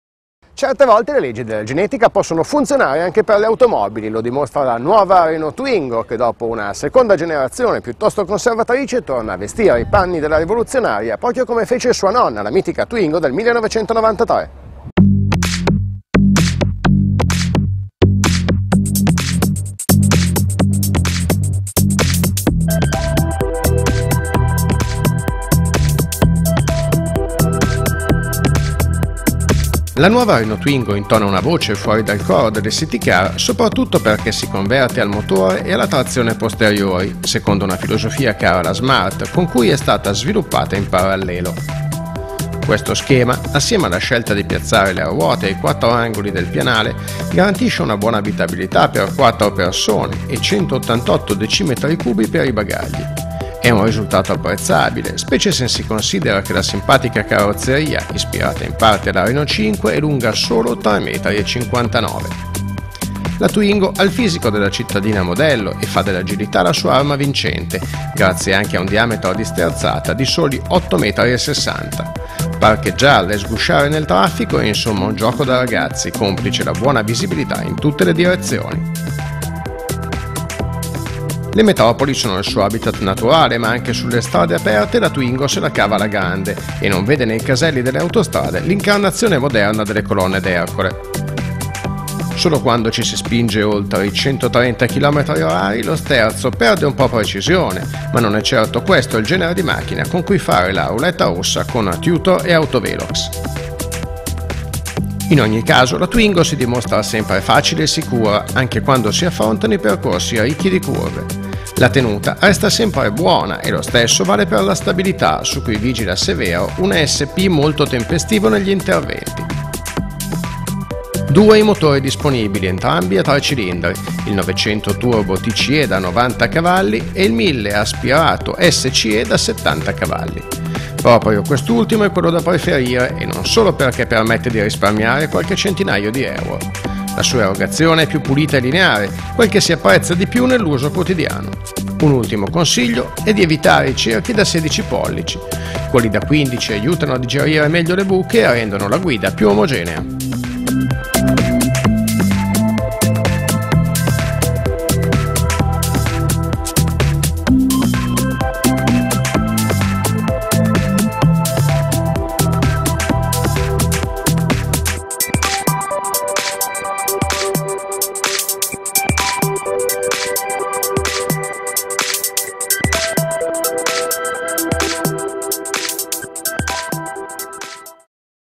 Nuova Renault Twingo
Con 5 porte e motore e trazione posteriori la Nuova Renault Twingo volta pagina rispetto al recente passato e torna in parte a vestire i panni della citycar rivoluzionaria che furono di sua nonna